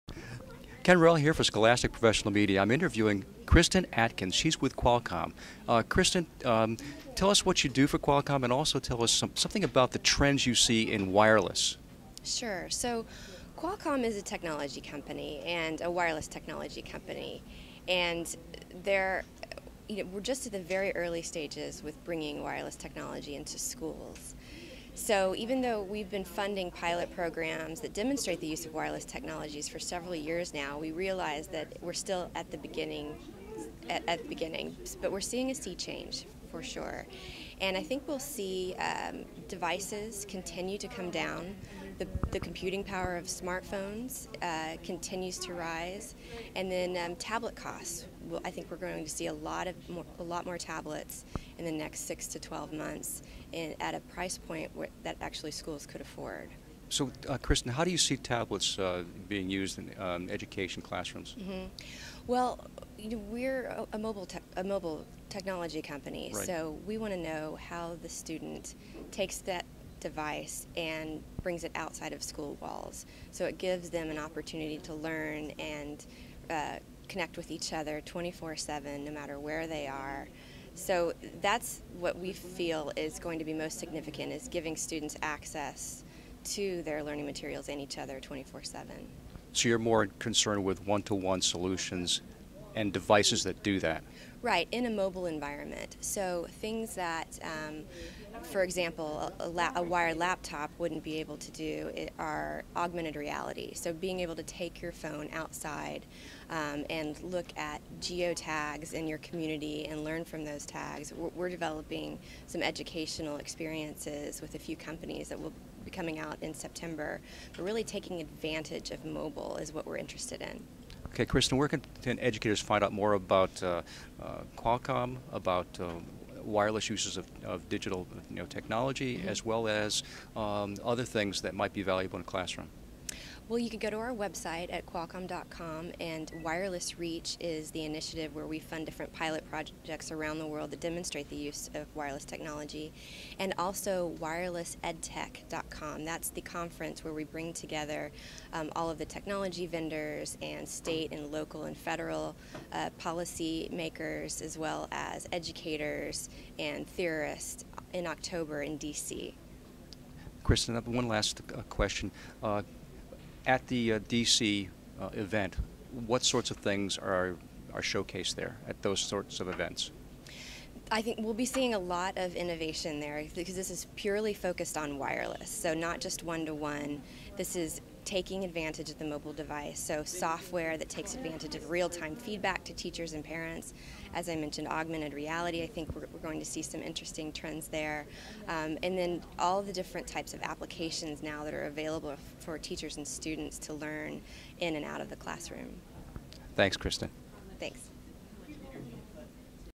Kristin Atkins Qualcomm ISTE.mov
Qualcomm's Kristin Atkins, Director of Wireless Reach, talks about tablets, wireless initiatives, and the D.C. Wireless Conference during our interview at ISTE Philadelphia.